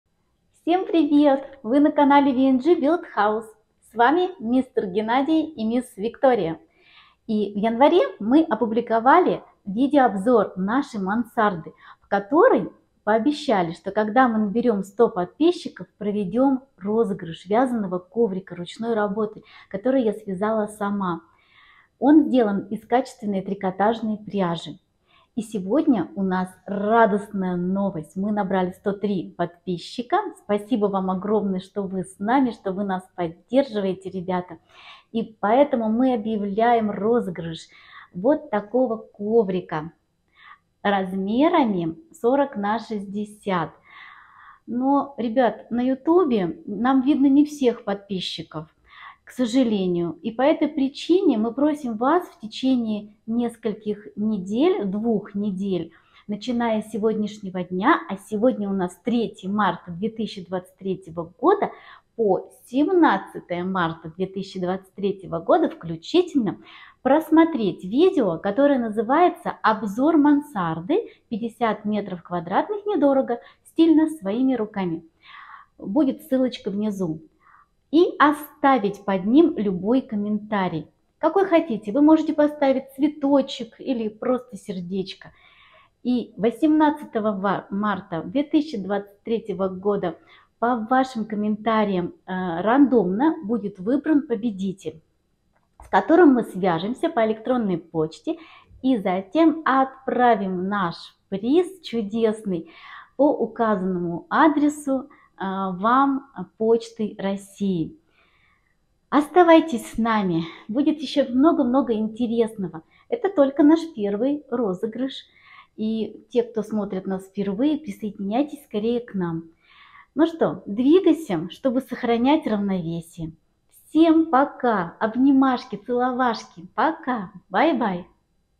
Внимание! Розыгрыш приза среди подписчиков!
Условия розыгрыша следующие:
 3) В период с  3 марта 2023 года по 17 марта 2023 года включительно  оставить под видео «Обзор мансарды 50 м. кв недорого, стильно, своими руками» любой комментарий.
4) 18 марта 2023 года по вашим комментариям рандомно через сайт youtogift.com будет выбран победитель, с которым мы свяжемся по электронной почте и затем отправим приз на указанный им адрес почтой России. А остальным ник победителя мы озвучим в итоговом видео.
Youtube никак не связан с данным розыгрышем и не несет ответственности за его проведение.